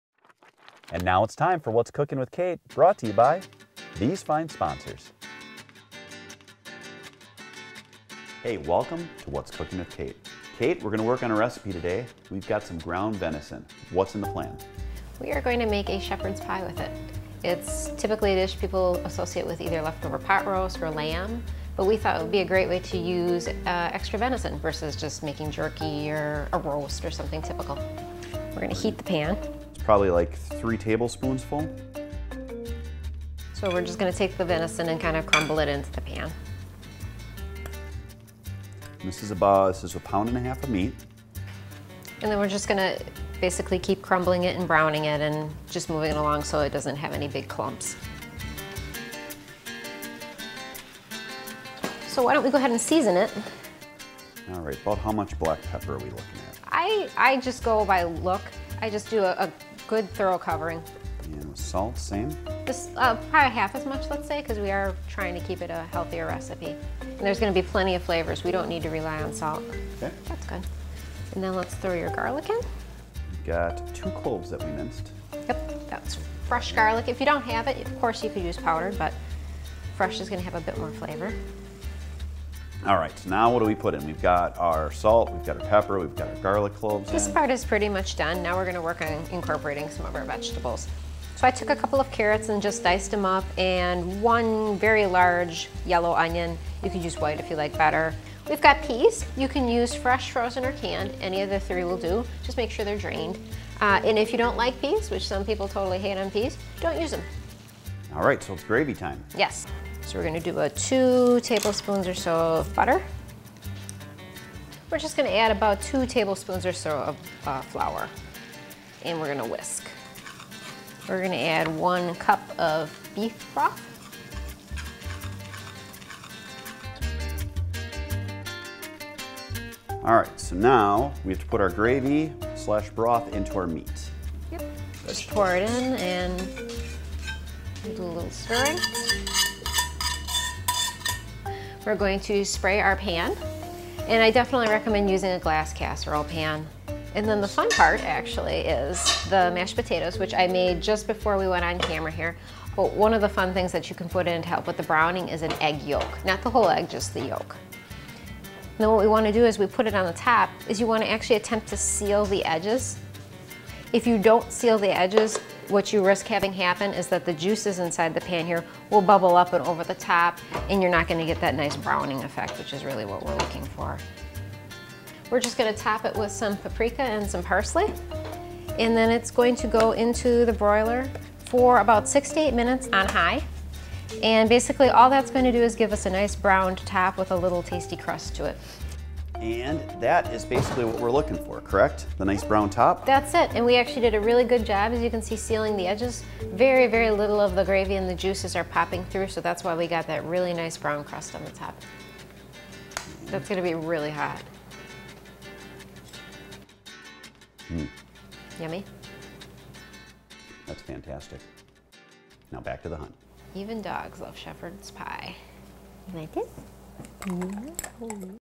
Simple and Easy Recipe Shepards Pie Recipe for your Deer Meat on What's Cooking with Kate
This episode of What's Cooking With Kate shows you how to make Shepards Pie with ground venison!  Simple and Easy that turns out downright awesome!